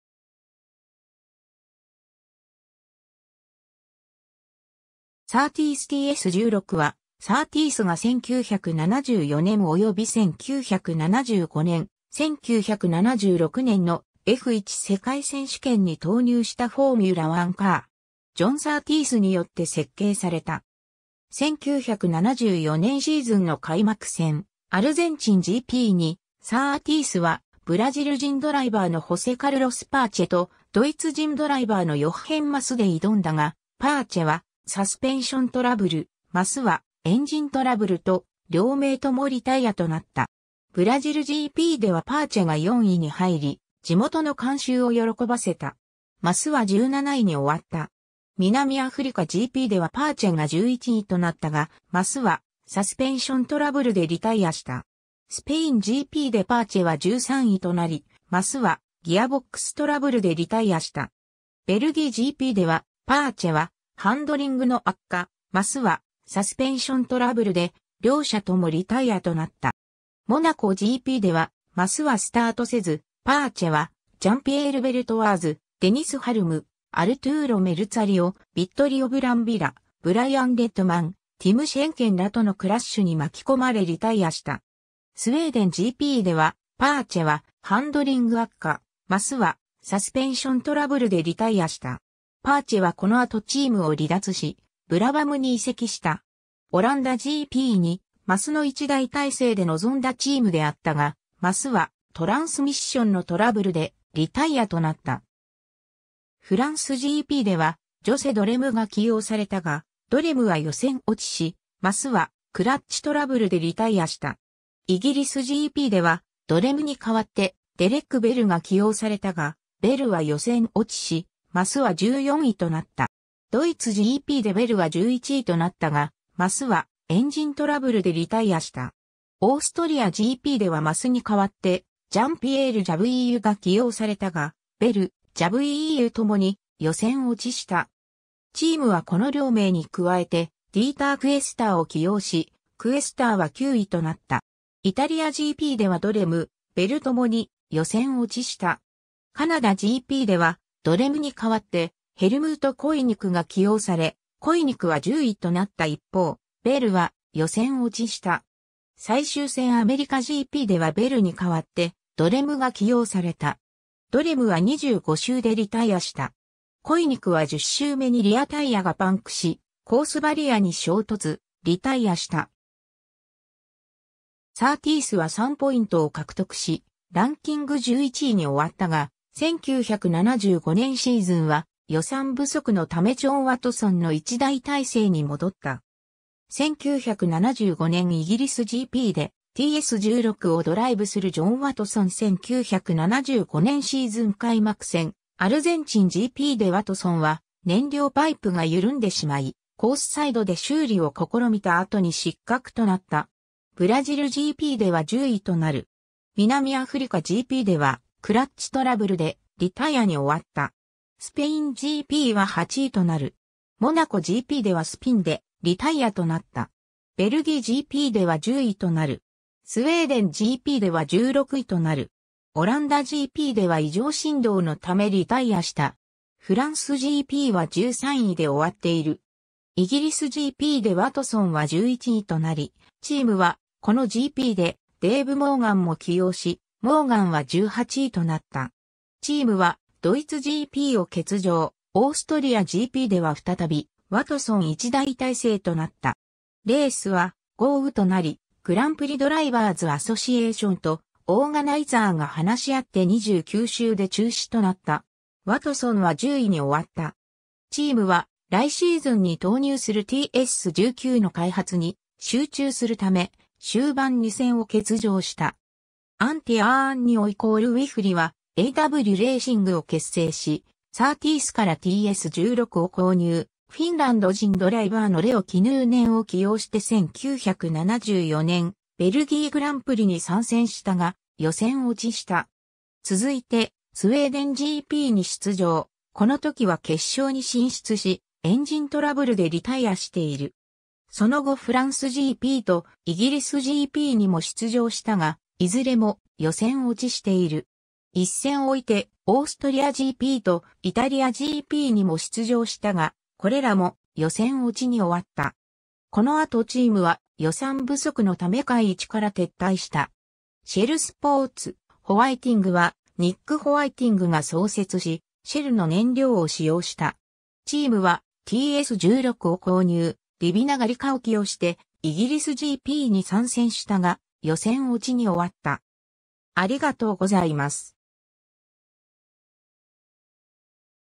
サーティース・TS16
サーティース・TS16 (Surtees TS16) は、サーティースが1974年および1975年、1976年のF1世界選手権に投入したフォーミュラ1カー。
ジョン・サーティースによって設計された 。
 1974年シーズンの開幕戦、アルゼンチンGPにサーティースはブラジル人ドライバーのホセ・カルロス・パーチェとドイツ人ドライバーのヨッヘン・マスで挑んだが、パーチェはサスペンショントラブル、マスはエンジントラブルと両名ともリタイアとなった。
ブラジルGPではパーチェが4位に入り、地元の観衆を喜ばせた。
マスは17位に終わった。
南アフリカGPではパーチェが11位となったが、マスはサスペンショントラブルでリタイアした。
スペインGPでパーチェは13位となり、マスはギアボックストラブルでリタイアした。
ベルギーGPではパーチェはハンドリングの悪化、マスはサスペンショントラブルで両者ともリタイアとなった。
モナコGPではマスはスタートせず、パーチェはジャン＝ピエール・ベルトワーズ（BRM）、デニス・ハルム（マクラーレン）、アルトゥーロ・メルツァリオ（イソ・マールボロ）、ヴィットリオ・ブランビラ（マーチ・エンジニアリング）、ブライアン・レッドマン（シャドウ）、ティム・シェンケン（トロージャン）らとのクラッシュに巻き込まれリタイアした。
スウェーデンGPではパーチェはハンドリング悪化、マスはサスペンショントラブルでリタイアした。
パーチェはこの後チームを離脱し、ブラバムに移籍した。
オランダGPにマスの1台体制で臨んだチームであったが、マスはトランスミッションのトラブルでリタイアとなった。
フランスGPではジョセ・ドレムが起用されたが、ドレムは予選落ちし、マスはクラッチトラブルでリタイアした。
イギリスGPではドレムに代わってデレック・ベルが起用されたが、ベルは予選落ちし、マスは14位となった。
ドイツGPでベルは11位となったが、マスはエンジントラブルでリタイアした。
オーストリアGPではマスに代わってジャン＝ピエール・ジャブイーユが起用されたが、ベル、ジャブイーユ共に予選落ちした。
チームはこの両名に加えてディーター・クエスターを起用し、クエスターは9位となった。
イタリアGPではドレム、ベル共に予選落ちした。
カナダGPではドレムに代わってヘルムート・コイニクが起用され、コイニクは10位となった一方、ベルは予選落ちした。
最終戦アメリカGPではベルに代わってドレムが起用された。
ドレムは25周でリタイアした。
コイニクは10周目にリアタイヤがパンクしコースバリアに衝突、リタイアした。
サーティースは3ポイントを獲得し、ランキング11位に終わったが、1975年シーズンは予算不足のためジョン・ワトソンの1台体制に戻った。
 1975年イギリスGPでTS16をドライブするジョン・ワトソン 1975年シーズン開幕戦アルゼンチンGPでワトソンは燃料パイプがゆるんでしまい、コースサイドで修理を試みた後に失格となった。
ブラジルGPでは10位となる。
南アフリカGPではクラッチトラブルでリタイアに終わった。
スペインGPは8位となる。
モナコGPではスピンでリタイアとなった。
ベルギーGPでは10位となる。
スウェーデンGPでは16位となる。
オランダGPでは異常振動のためリタイアした。
フランスGPは13位で終わっている。
イギリスGPでワトソンは11位となり、チームはこのGPでデイヴ・モーガンも起用し、モーガンは18位となった。
チームはドイツGPを欠場、オーストリアGPでは再びワトソン1台体制となった。
レースは豪雨となり、グランプリ・ドライバーズ・アソシエーションとオーガナイザーが話し合って29周で中止となった。
ワトソンは10位に終わった。
チームは来シーズンに投入するTS19の開発に集中するため終盤2戦を欠場した。
 アンティ・アーニオ＝ウィフリはAAWレーシングを結成し、サーティースからTS16を購入、フィンランド人ドライバーのレオ・キヌーネンを起用して1974年ベルギーグランプリに参戦したが、予選落ちした。
続いてスウェーデンGPに出場、このときは決勝に進出しエンジントラブルでリタイアしている。
その後フランスGPとイギリスGPにも出場したが、いずれも予選落ちしている。
一戦おいてオーストリアGPとイタリアGPにも出場したが、これらも予選落ちに終わった。
こ...